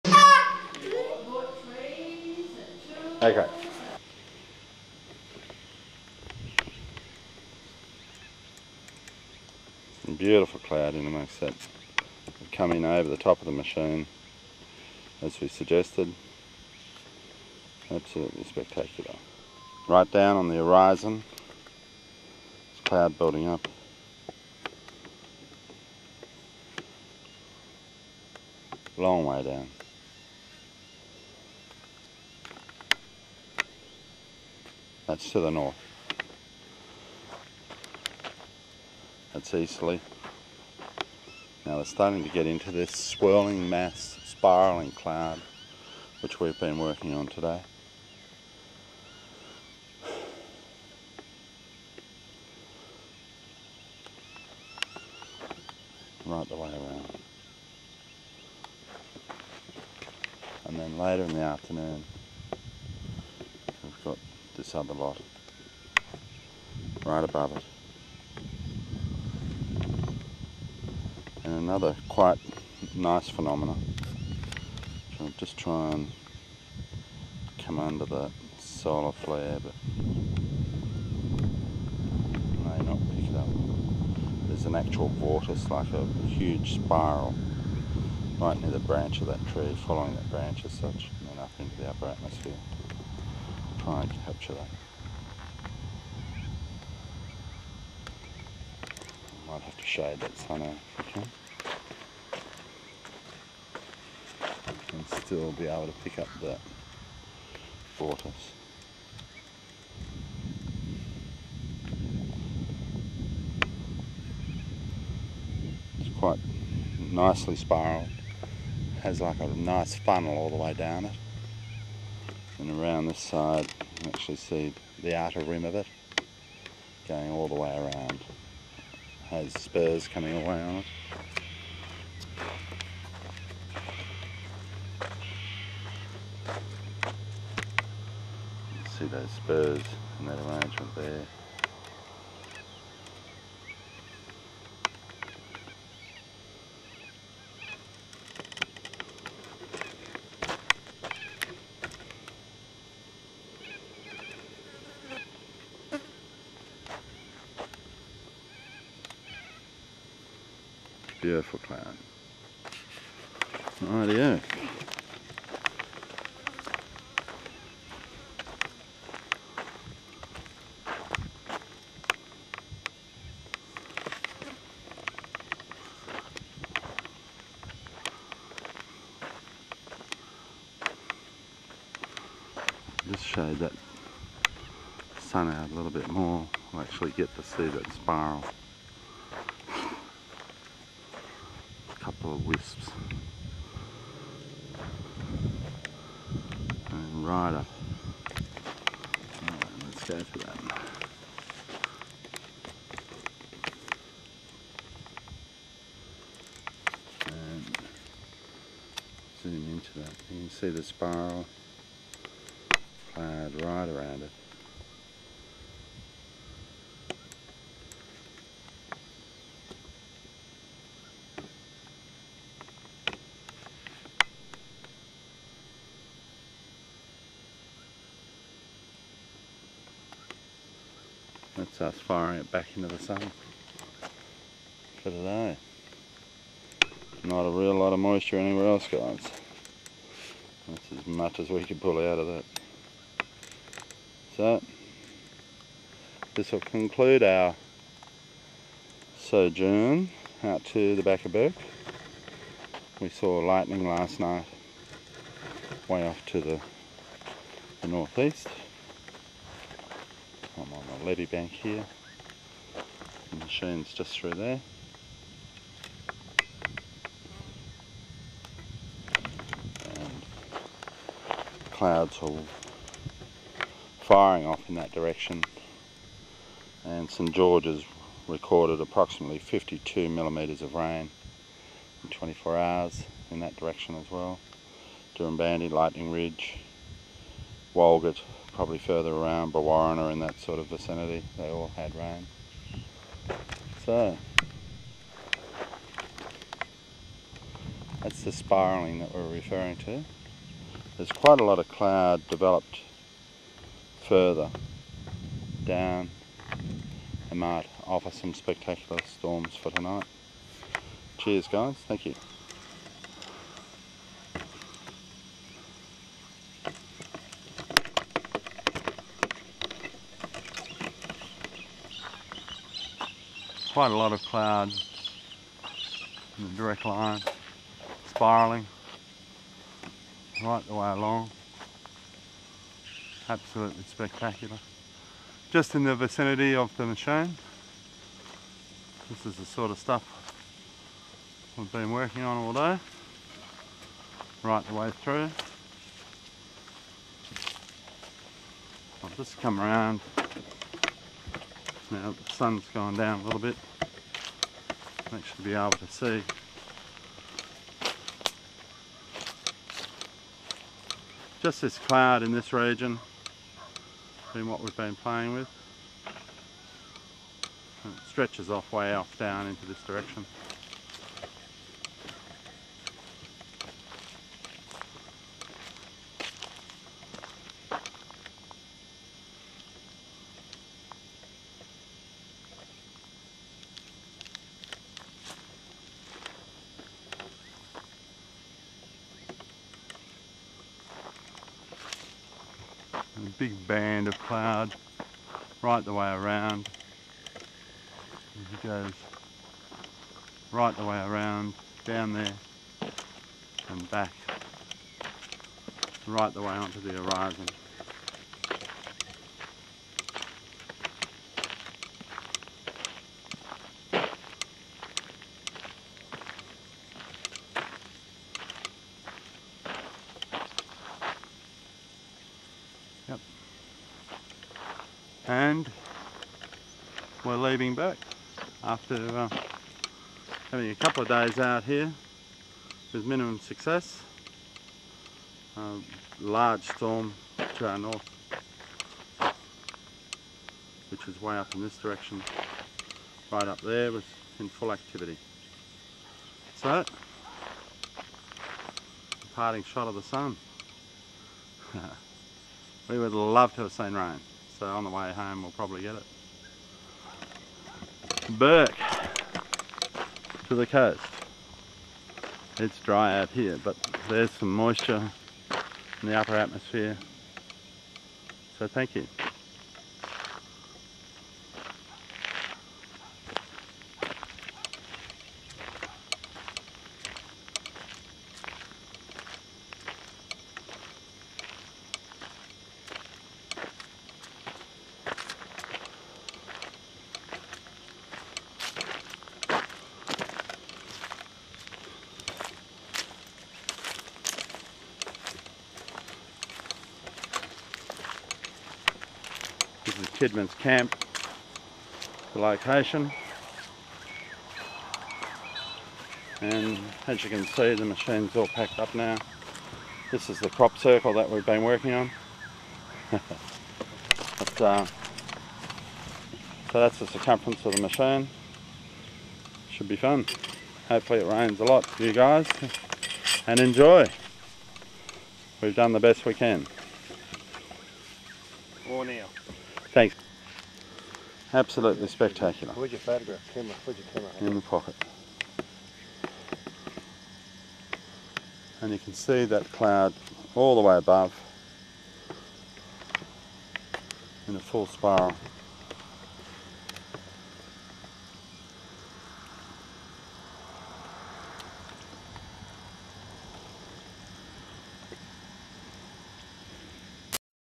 Atmospheric Ionization Rain from Bourke NSW Australia 09 Jan 2007
Atmospheric Ionization Rain
Experimenting using a range of frequencies causing Rain